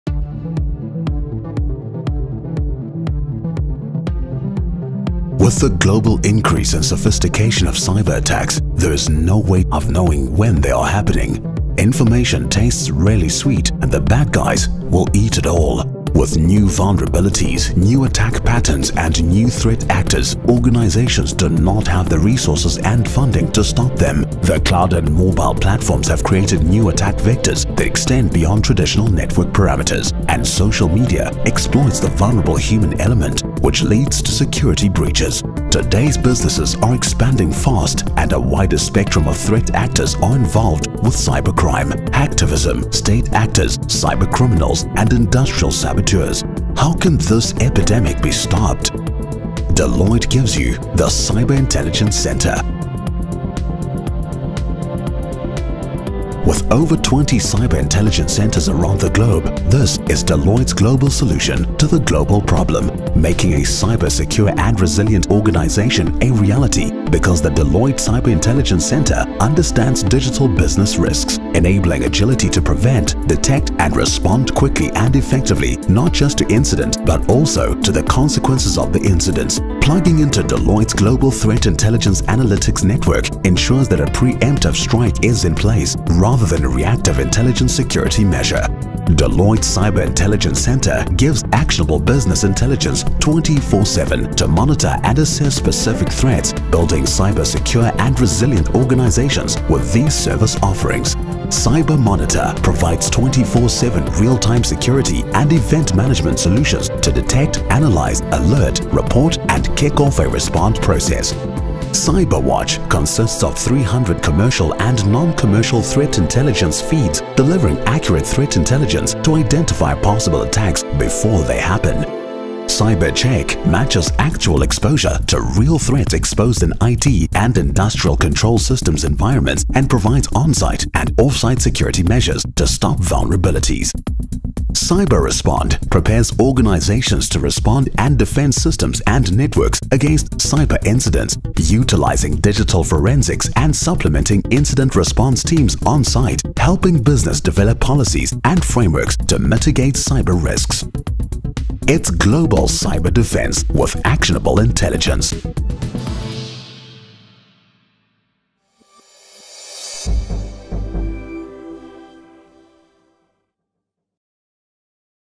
Cyber Intelligence Centre - Fusion framework
Deloitte transforms cyber security information into tangible intelligence through +300 threat intelligence feeds, assess these threats specific to your organisation on a 24x7 basis, enabling effective detection, identification and mitigation of potential cyber attack to transform your business into a vigilant enterprise.